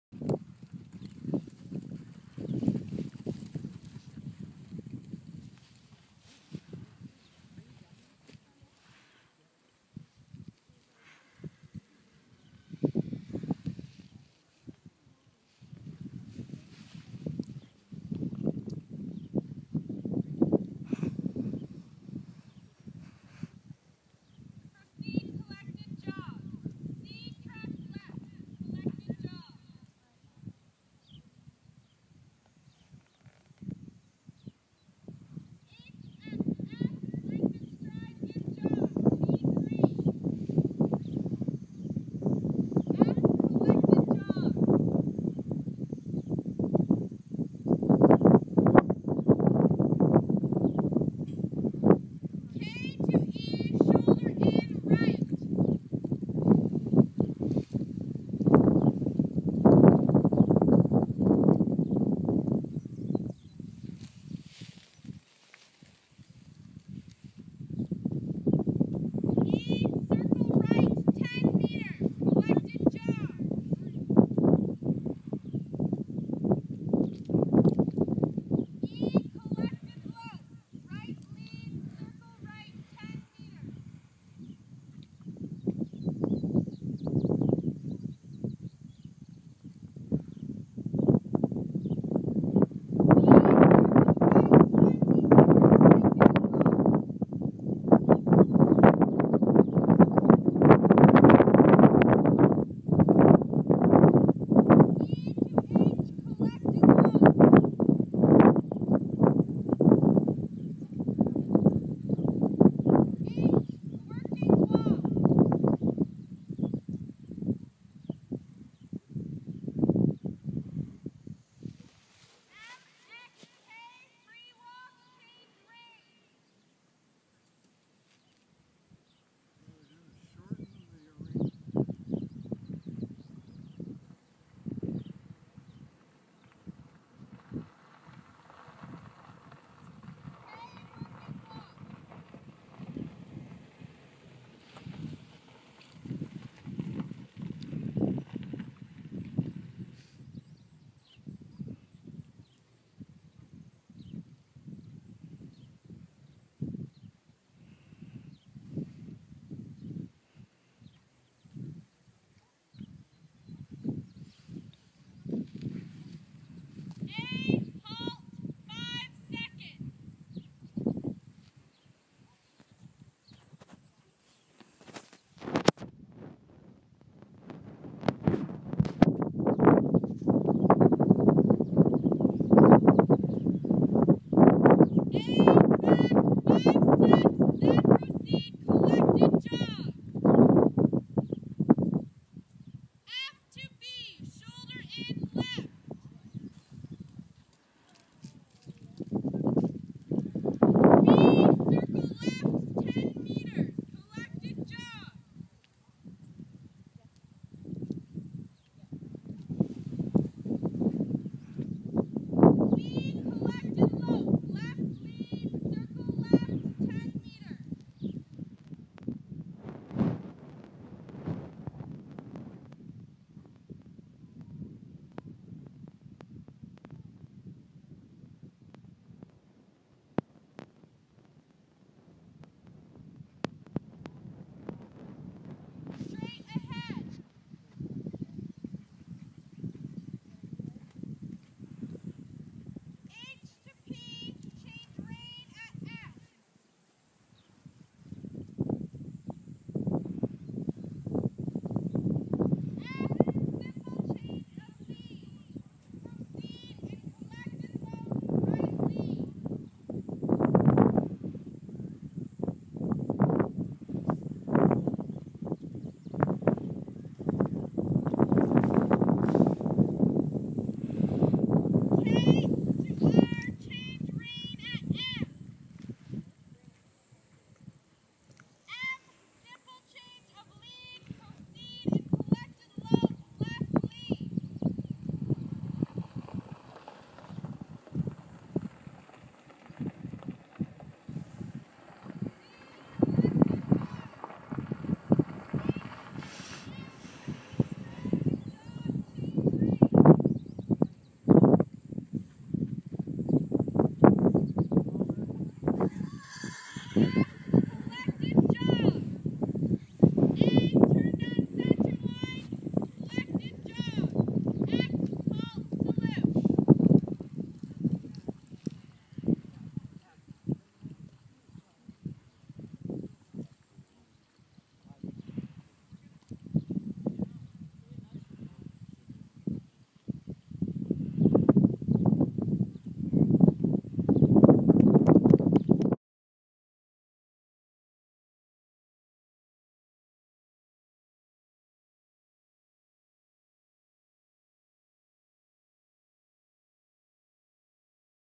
20140809 Stellar Mount Schooling Show - Level 2 Test 4 - 64.6%
Stellar Mount Stables Schooling Show Aug 9, 2014 in Port Lambton, Ontario.  Western Dressage Level 2 Test 4. Sexy Superstar and Jennifer Postma.